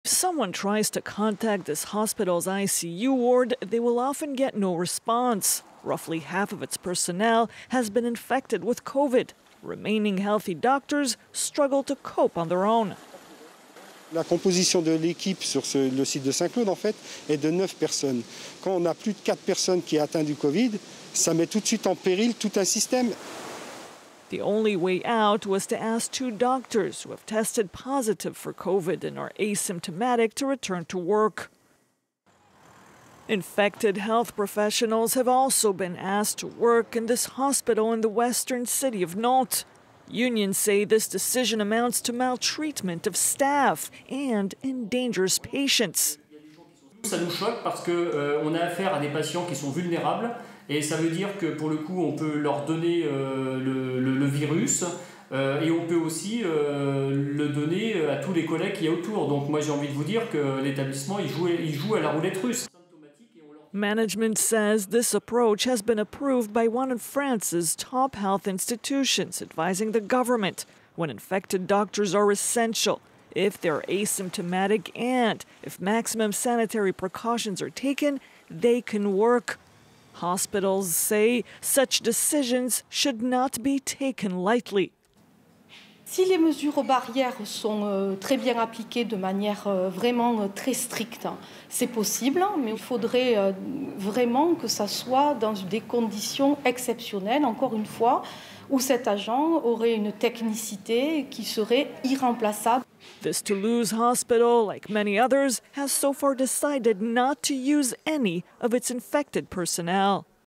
'Management is playing the russian roulette': When doctors infected with Covid-19 continue to work
People who have been infected with Covid-19 are usually asked to stay home and quarantined. But that rule does not seem to apply to everyone: some hospitals in France have made some of their staff come to work even if they had been infected, because they have no one to replace them. Those doctors and nurses are asymptomatic but some say health institutions are putting patients at risk.